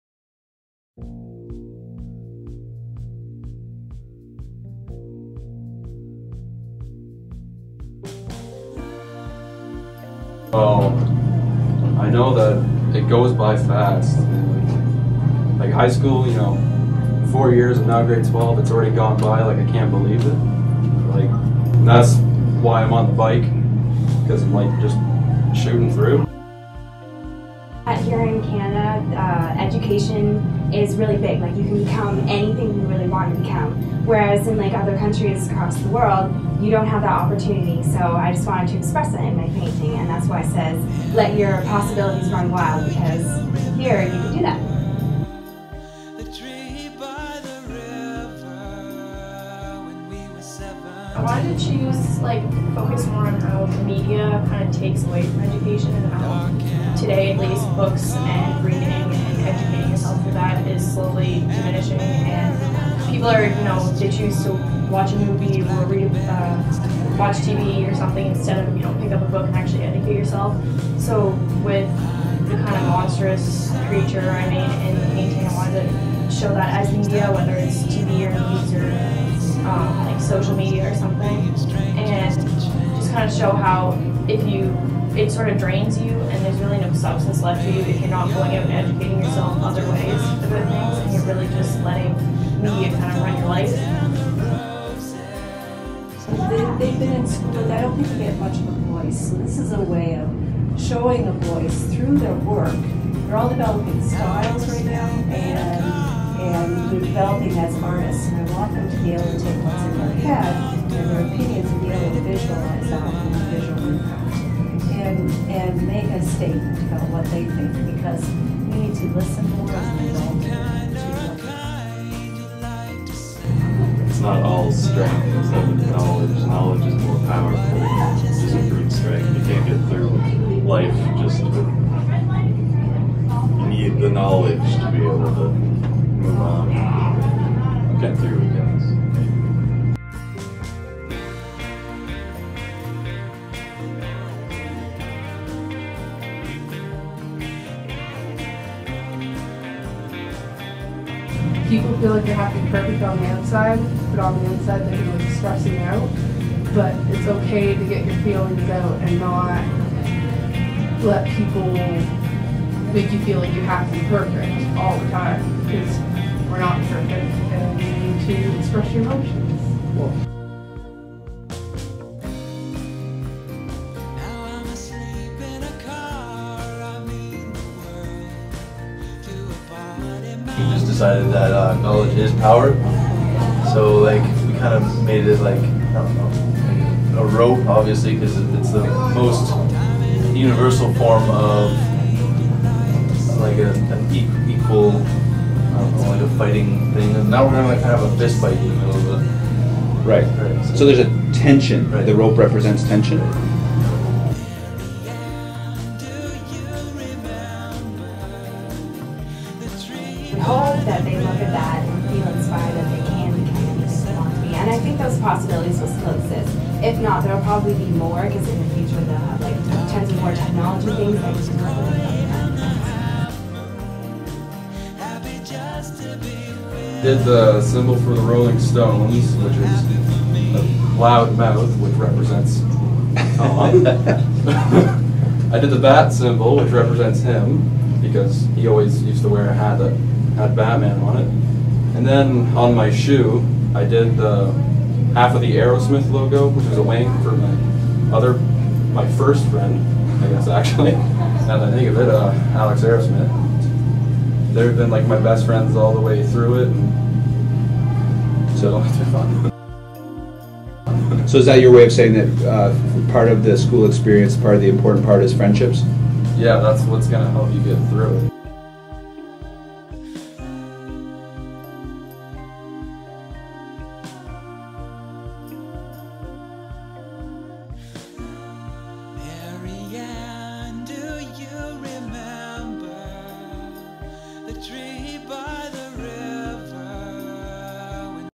Grade 12 Visual Art: The Power Of Education
In Semester 1 of 2013/14 Mrs. Belanger challenged her grade 12 visual arts class to create paintings that express the 'power of education.'  We then installed the works in the hall and captured the artists' comments here.